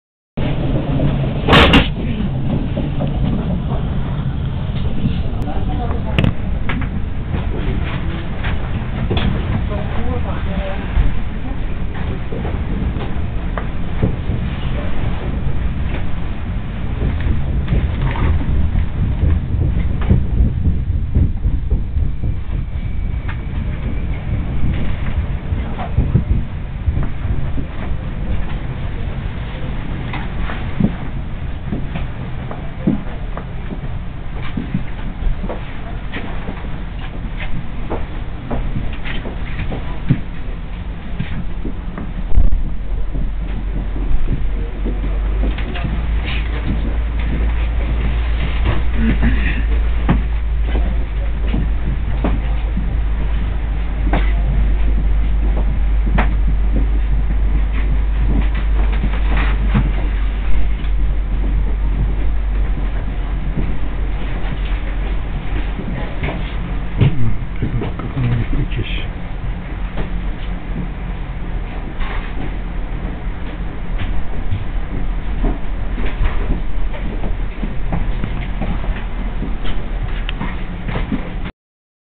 Aankomst sneltrein in Hoorn (Hoorn CS).
De aankomst van de sneltrein in de stad Hoorn: de forenzen stappen uit